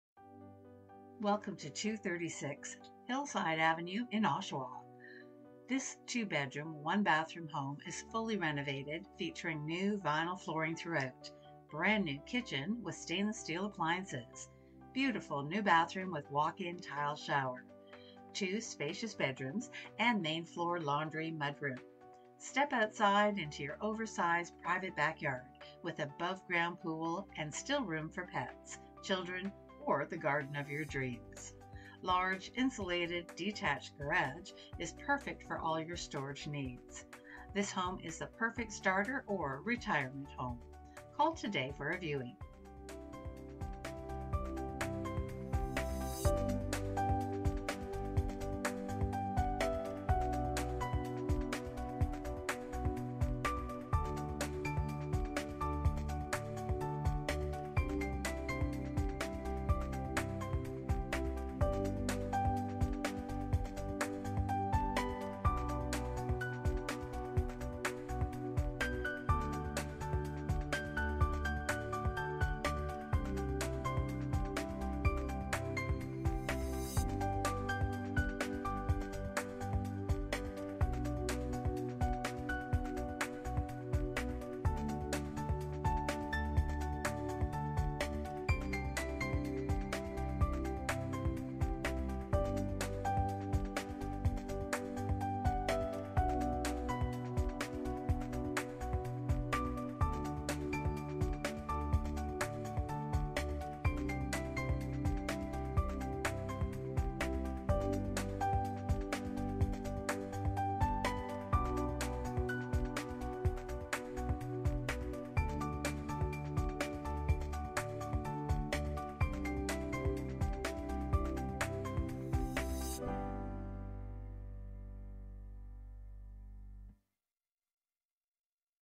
236 Hillside Avenue, Oshawa
Welcome to 236 Hillside Ave in Oshawa! This 2 bedroom, 1 bathroom home is fully renovated featuring new vinyl flooring throughout, brand new kitchen with stainless steel appliances, beautiful new bathroom with walk in tile shower, 2 spacious bedrooms and main floor laundry/mudroom. Step outside into your oversized private backyard with above ground pool and still room for pets, children or the garden of your dreams. Large insulated detached garage is perfect for all your storage needs. This home is the perfect starter or retirement home.

Sarah Hill
Century 21 Infinity Realty Inc.
211- 650 King Street East
Oshawa, L1H 1G5